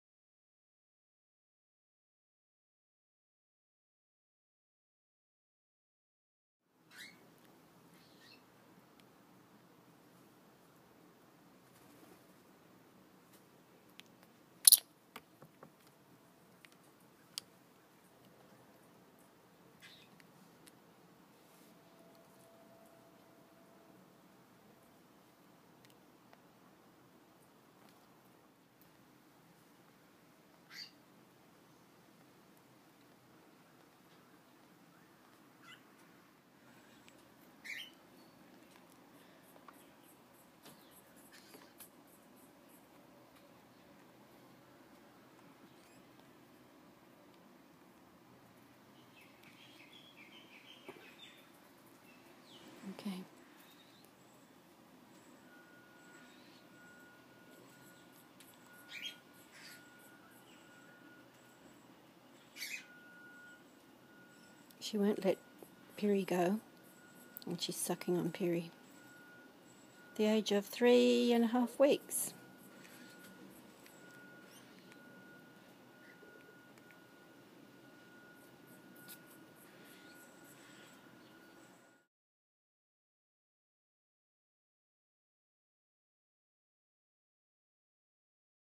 Sucking behaviour in baby Flying-foxes, 2
Sometimes hand raised baby flying-foxes display an aberrant behaviour called "sucking".  The neediest pup, the sucker, will latch onto the other pup, the suckee, and either suck up tufts of fur on the back of the suckee's neck, of their ear, or even their penis sometimes.  They may also attempt to suckle under the suckee's wings and bring up love bites on their chest wall.
The pups will be permanently separated.
This behaviour seems to occur more frequently in times of great stress - when the carer is overloaded with too many pups, when there is a continual revolving door of pups coming in for stabilisation and going out to new carers.
Splitting up the pups causes more stress, but less physical and emotional longterm damage to the pups in question.